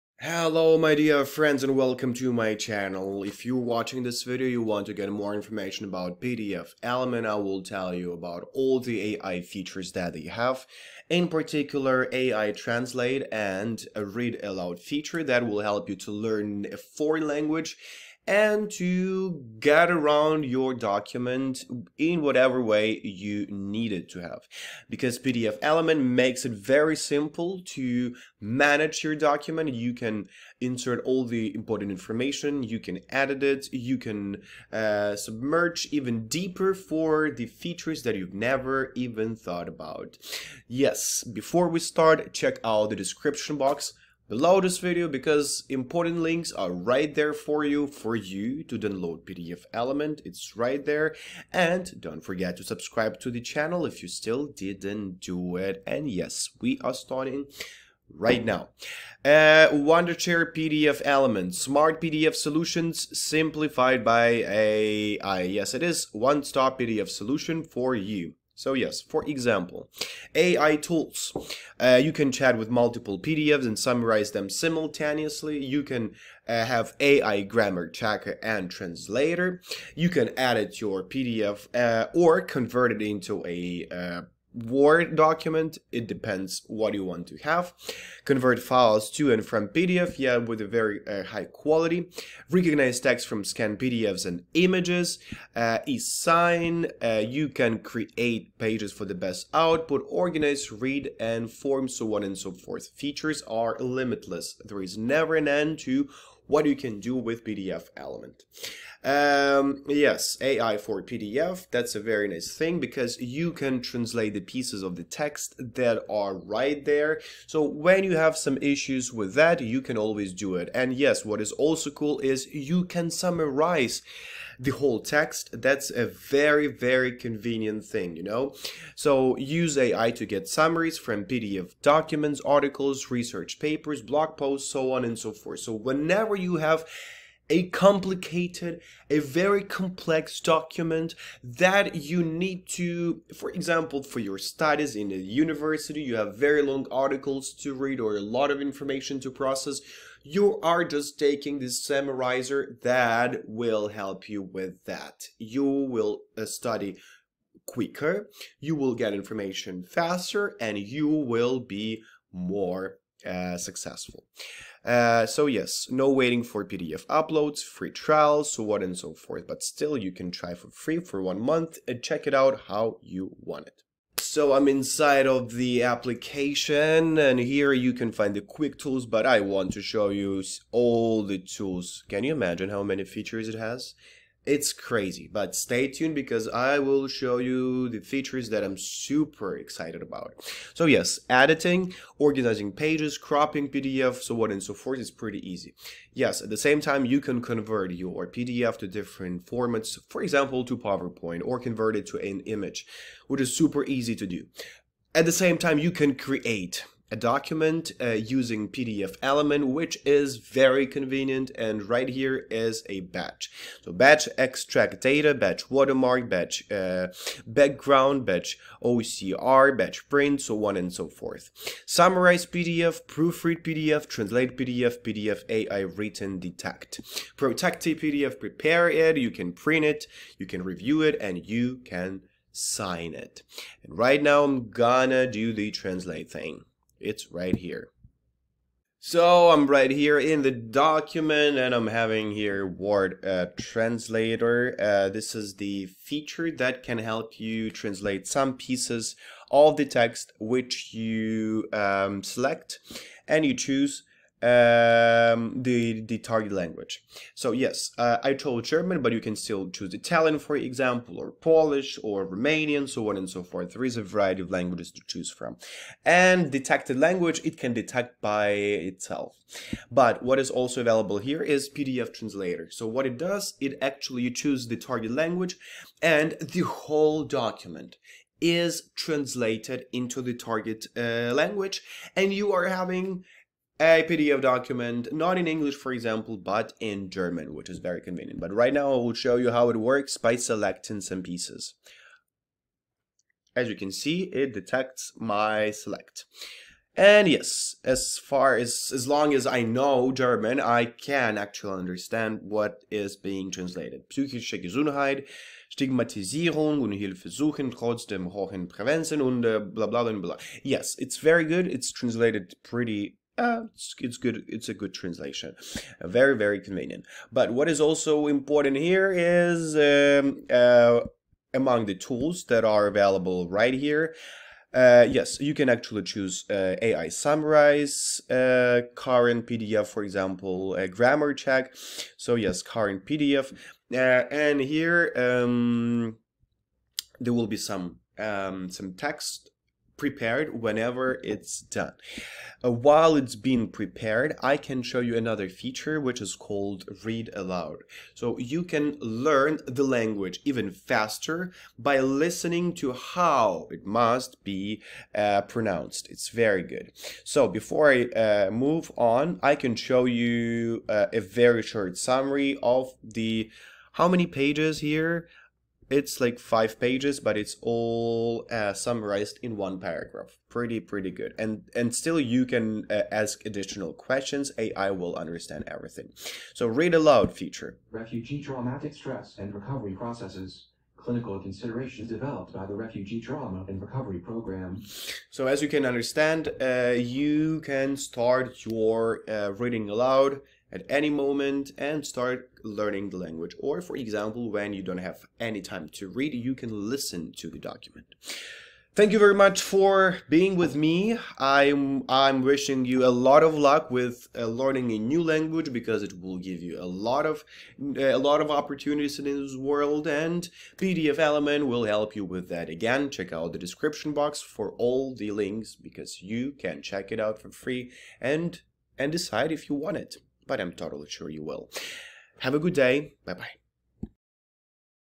Unleash AI Magic: Transform Your PDFs Instantly with Wondershare PDFelement
🔥 Step into a new era of PDF management with Wondershare PDFelement.
In this video, you’ll explore cutting-edge AI features that let you edit,
convert, and manage your PDFs in ways you never imagined. 🚀

- Experience a fresh approach to document handling that’s tailored
for both professionals and everyday users!

Key Moments:
• 🔹 0:00 – Introduction: Your journey begins
• 🔹 0:45 – Overview: Discover standout features
• 🔹 2:00 – AI Translation & Editing: Smart editing reimagined
• 🔹 3:45 – PDF Conversion: Power-packed conversion tools
• 🔹 5:15 – Mobile Integration: Flawless performance on the go
• 🔹 6:15 – Conclusion: Recap & exclusive offer

Elevate your PDF game and redefine productivity today! 🚀